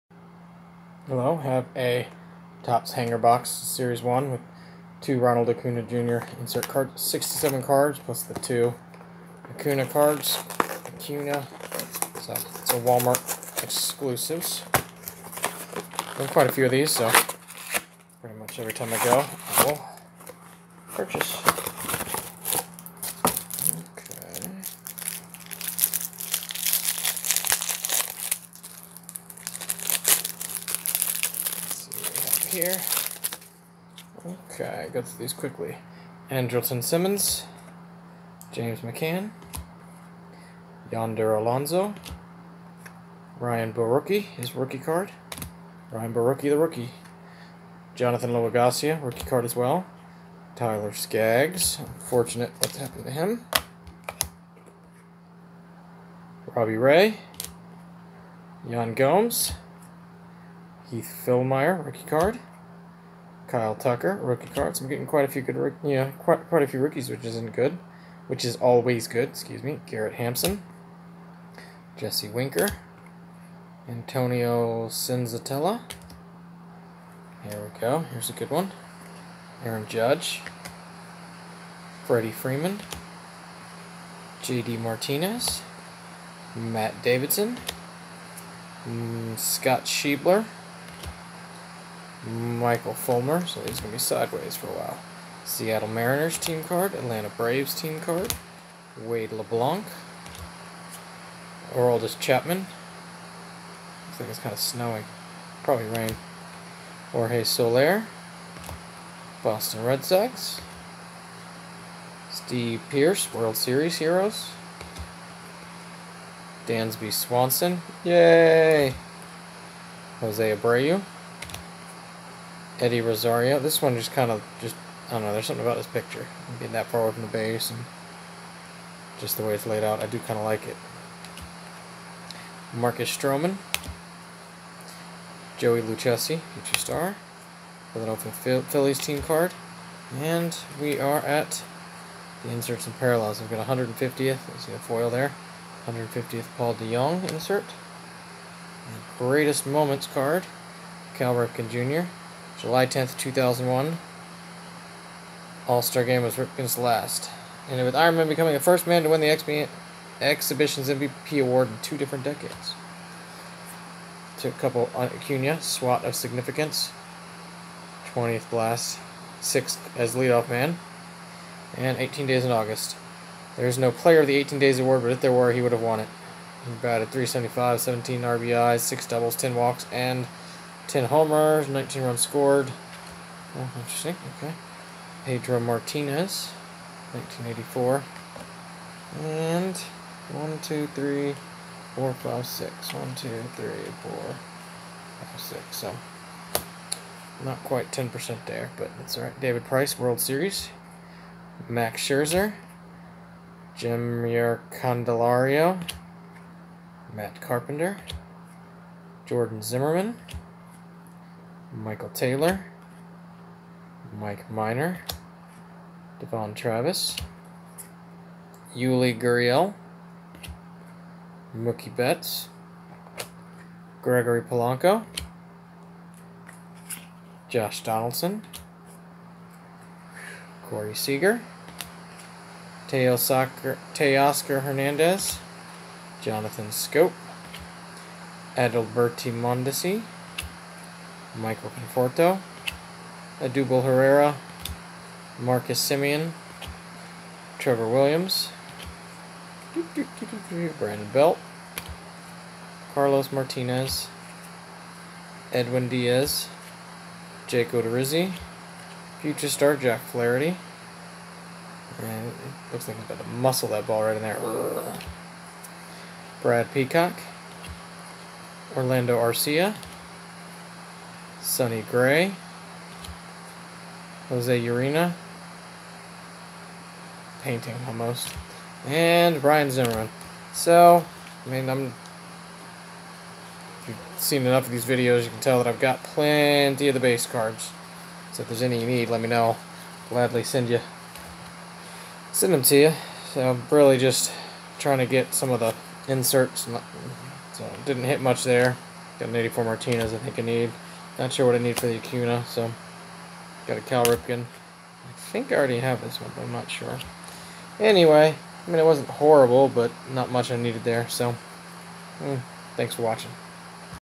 2019 Topps Series 1 - Hanger Box Break #15 (with Walmart Exclusive Ronald Acuna Highlights)
Opening a hanger pack of 2019 Topps Series 1 Hanger Box (67 Cards - 2 Walmart Exclusive Ronald Acuna Jr. Highlight Cards)